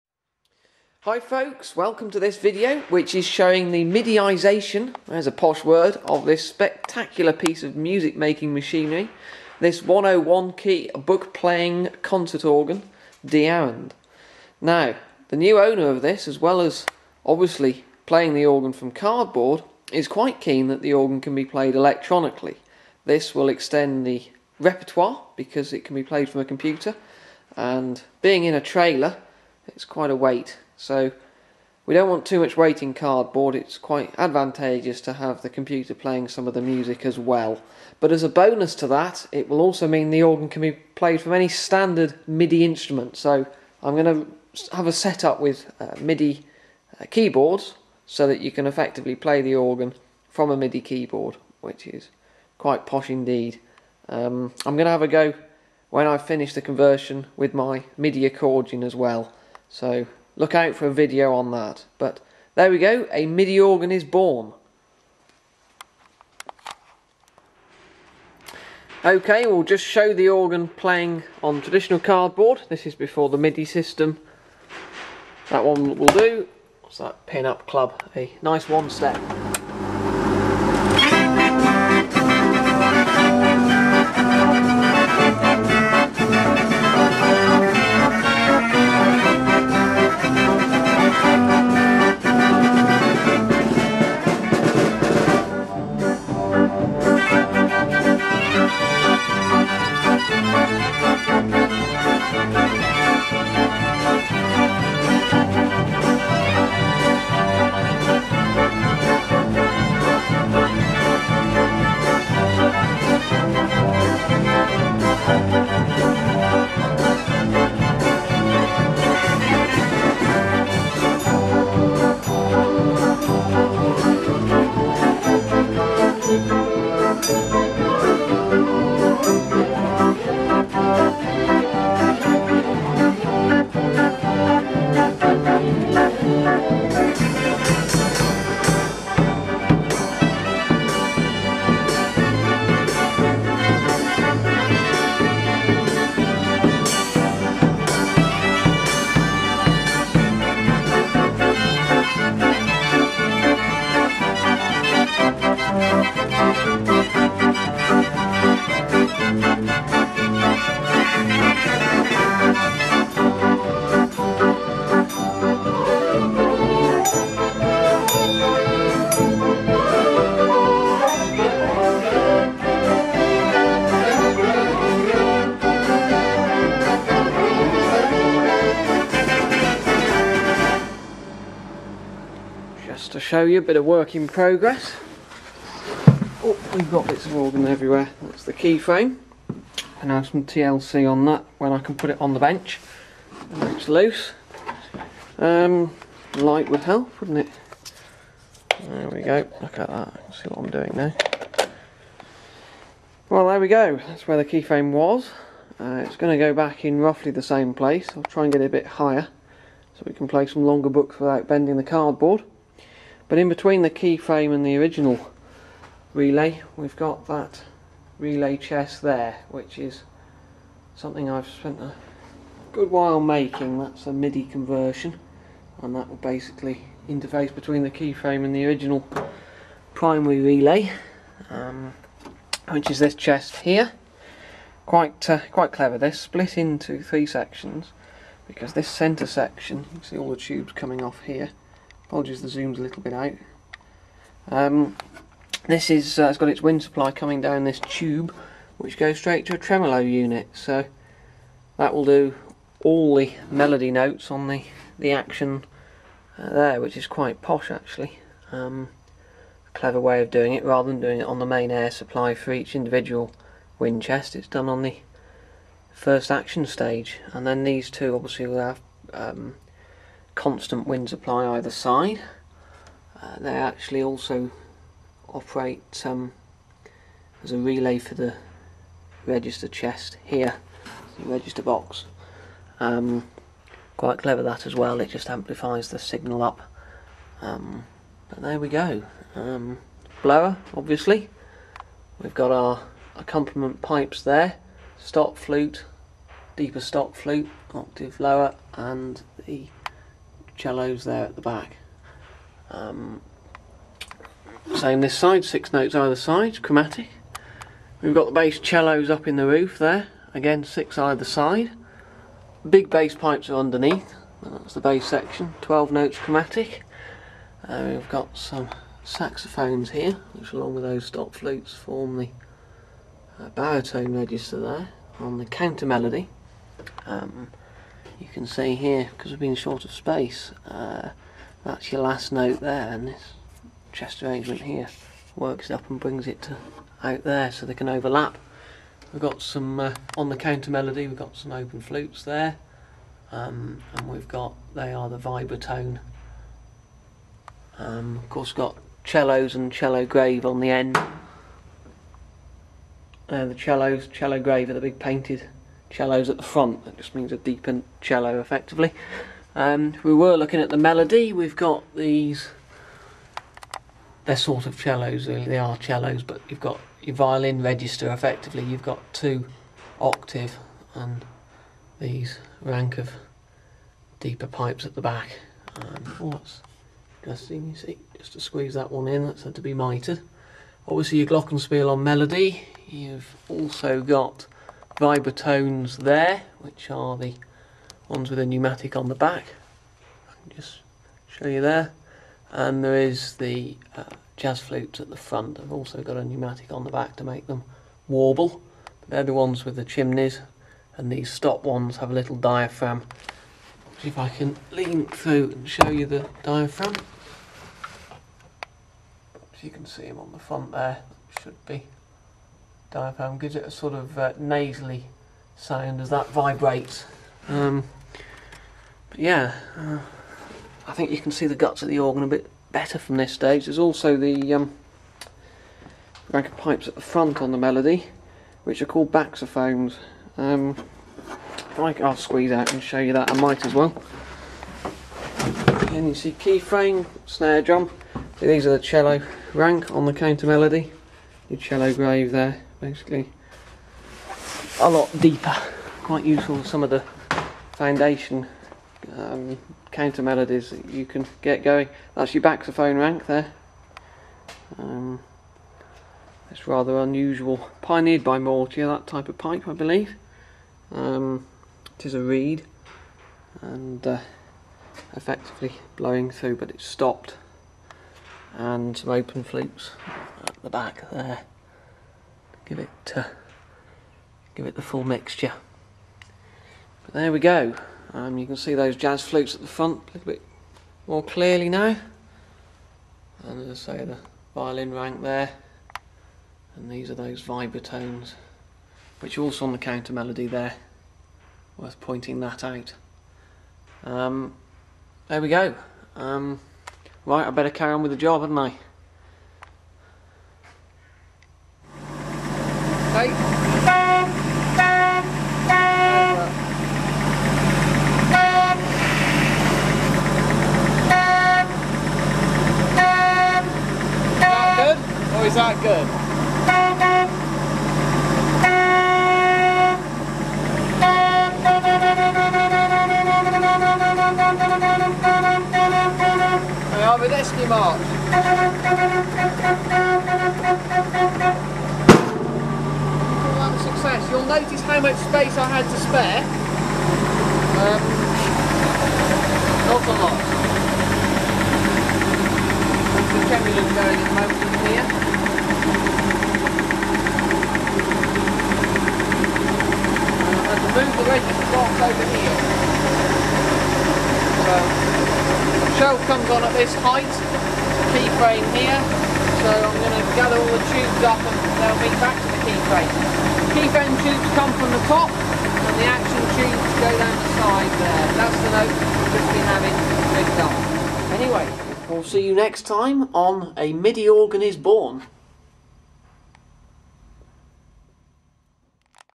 New MIDI System on 101-key Concert Organ "De Arend" 1
A MIDI organ is born! This is video 1 of 2 showing me fitting a new MIDI playing system to the 101-key Mortier scale Heesbeen Concert Organ "De Arend". My system still allows the organ to be played in the traditional way with perforated cardboard music read pneumatically by a device called the 'key frame', but now a computer MIDI player can also run the organ, or for that matter any standard MIDI output device (see my other videos for a demo of my playing the organ using my MIDI piano accordion!).